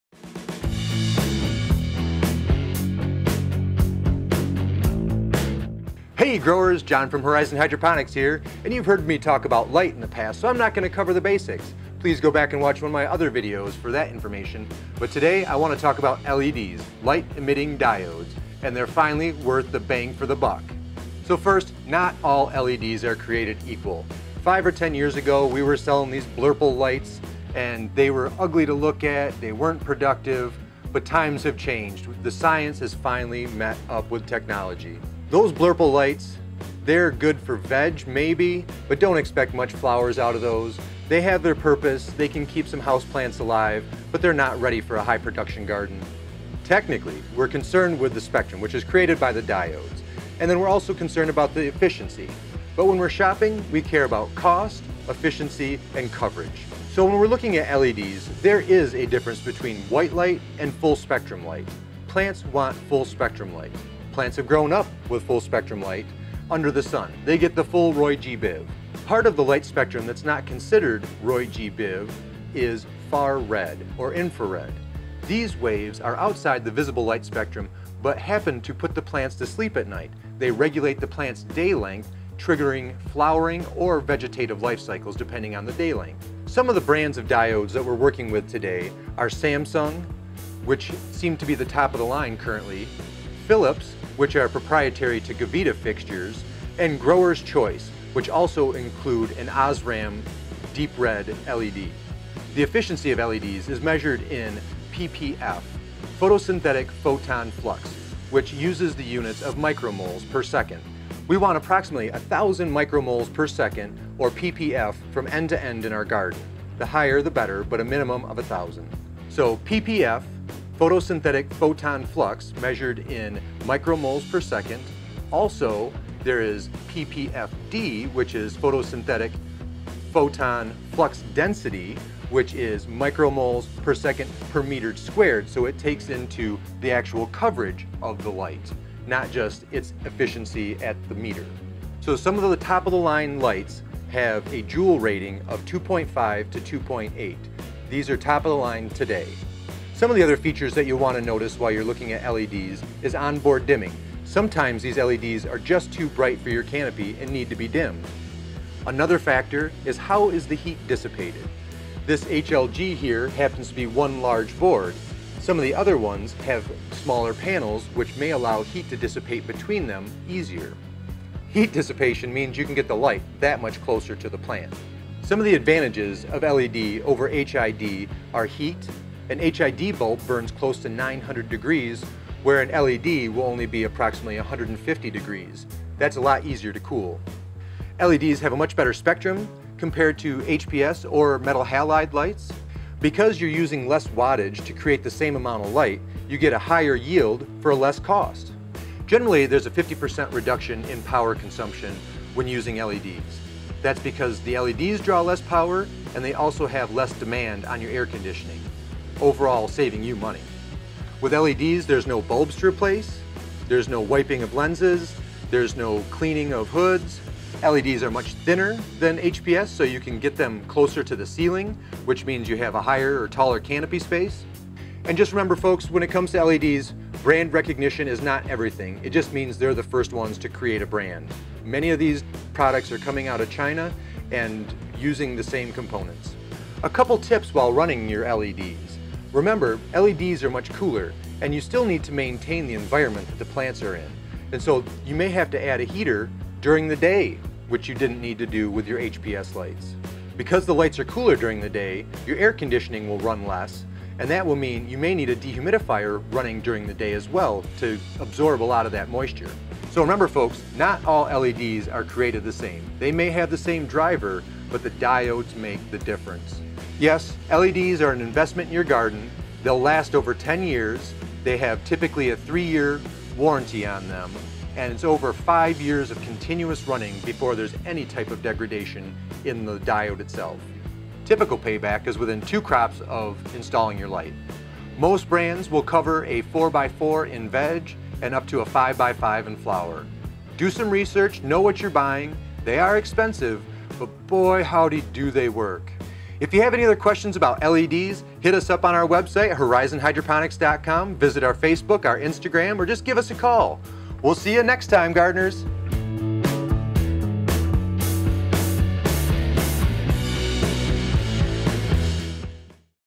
LED: Secrets Revealed
LEDs have come a long way since they were first introduced to the grow scene. Come take a look at what's new, what's hot and what's what!